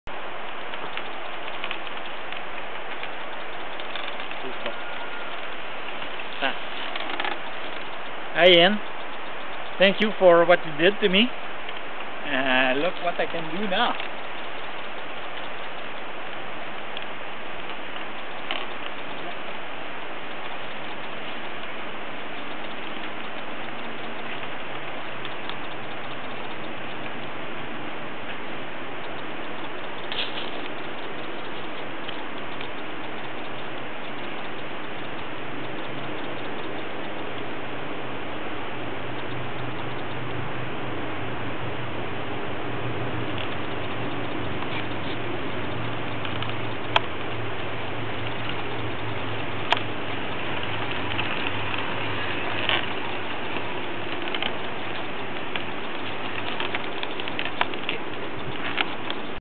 Jean Potvin Segway
Paraplégique à la hauteur de T5 il fait du segway